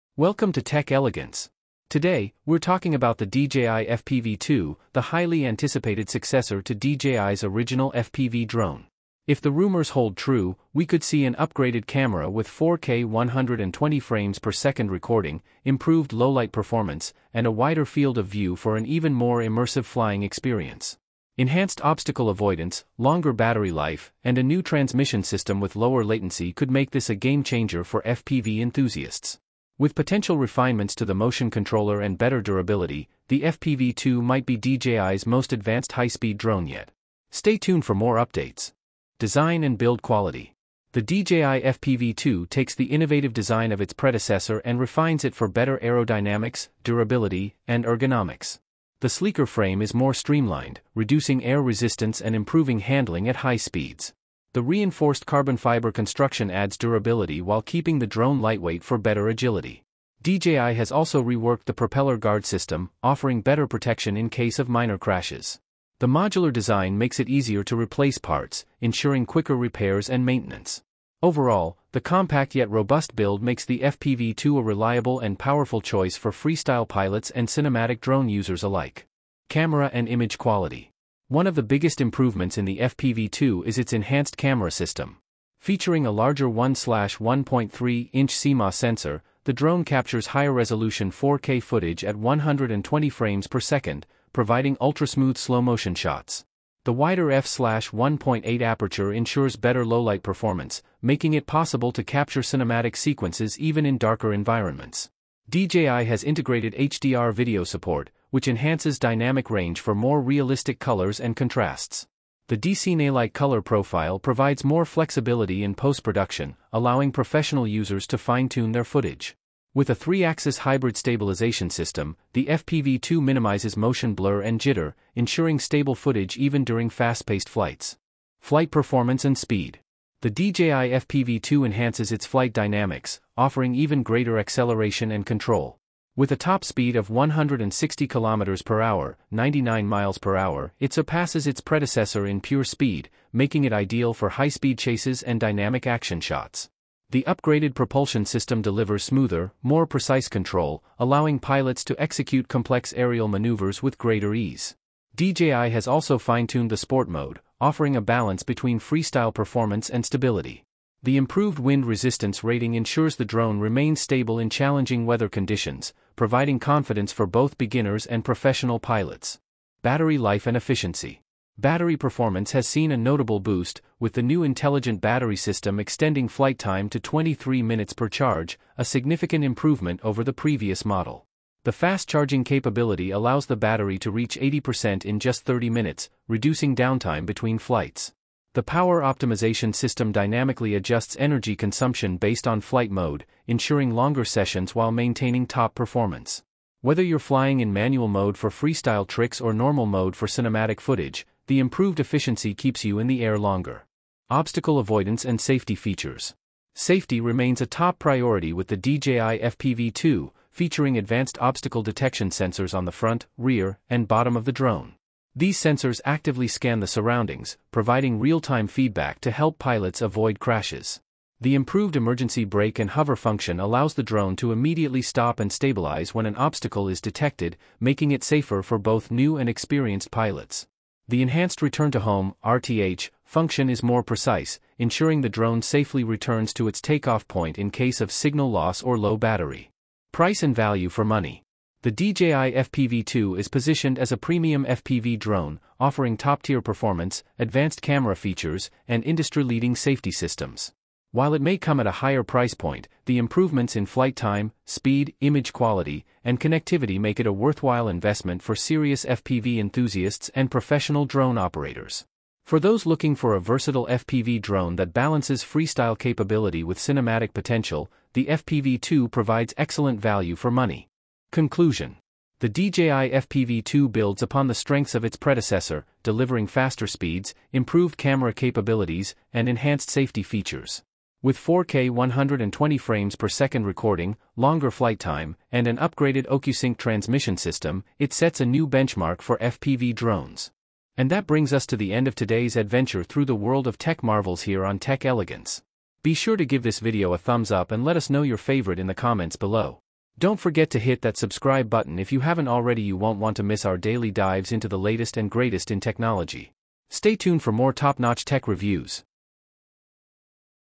DJI FPV 2: Leaked Specs, Release Date & First Look!
The DJI FPV 2 is set to revolutionize the world of first-person drone flying with cutting-edge technology and an immersive experience like never before. Featuring ultra-low latency transmission, improved flight stability, and a high-resolution FPV feed, this drone is designed for both adrenaline-fueled racers and cinematic content creators. With an upgraded camera system, enhanced battery life, and advanced obstacle avoidance, the DJI FPV 2 offers smoother flights and breathtaking aerial footage. Take your FPV experience to new heights with this next-generation drone from DJI!